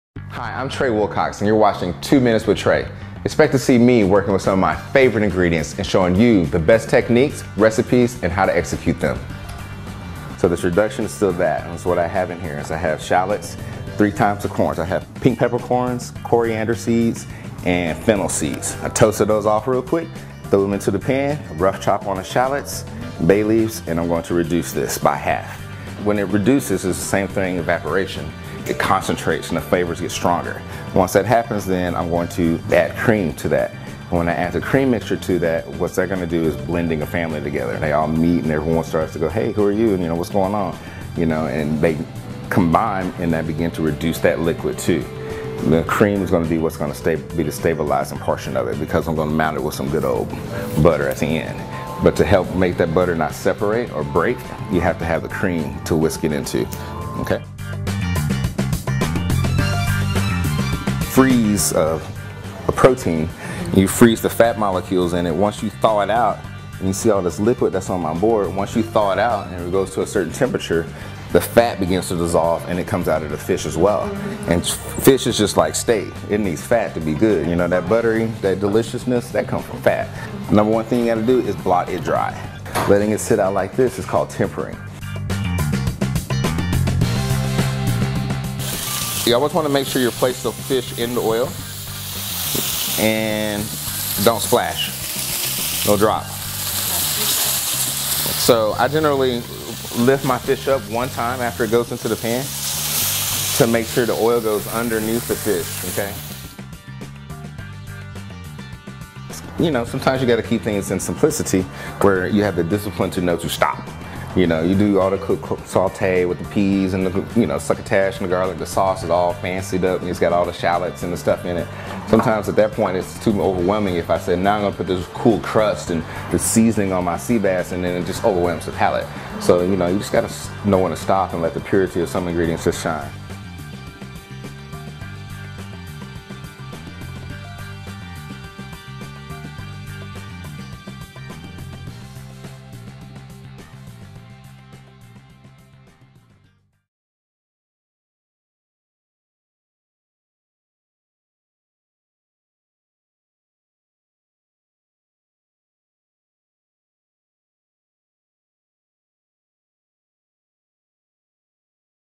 2 Minutes With Tre Wilcox - Crispy Seared Sea Bass
Having trouble cooking fish? Learn some great fish cooking techniques and how to master cooking a very popular fish...Got two minutes?